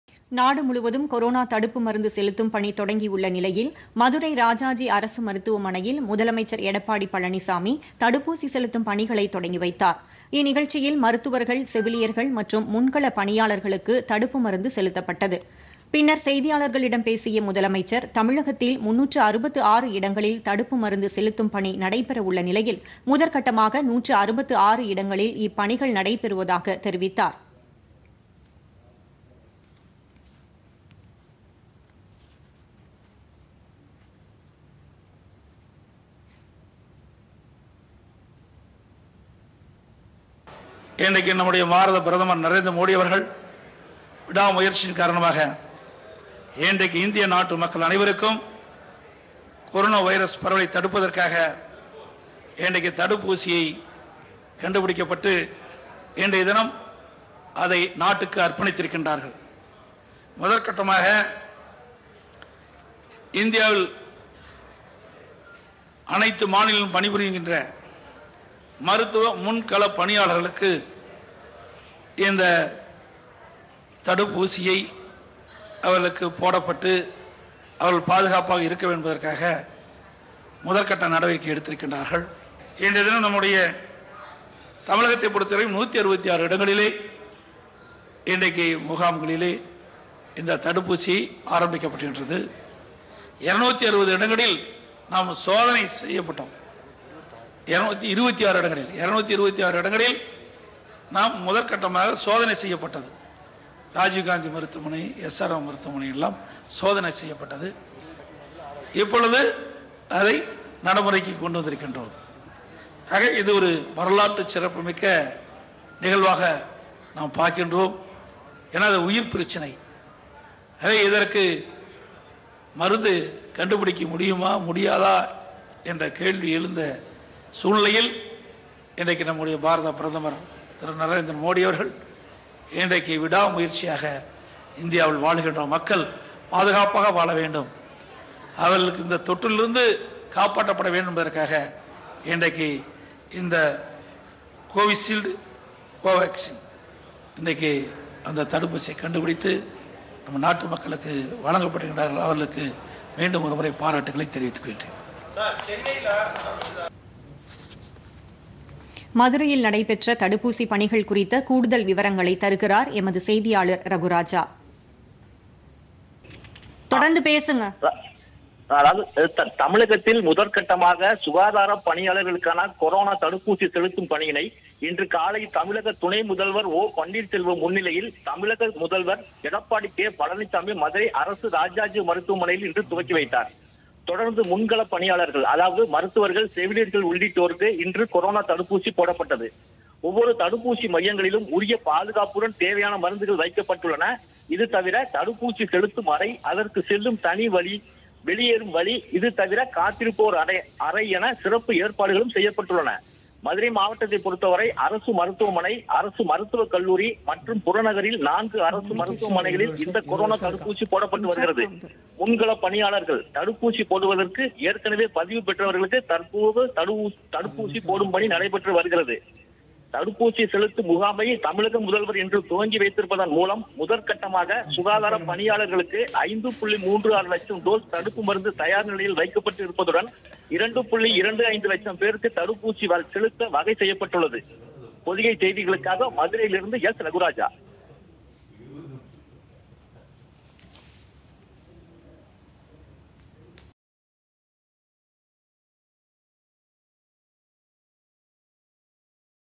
166 மையங்களில் தடுப்பு மருந்து செலுத்தும் பணி
தமிழகத்தில் 166 மையங்களில் கொரோனா தடுப்பு மருந்து செலுத்தும் பணி  - மதுரை அரசு மருத்துவமனையில் தொடங்கி வைத்தார் முதலமைச்சர் எடப்பாடி பழனிசாமி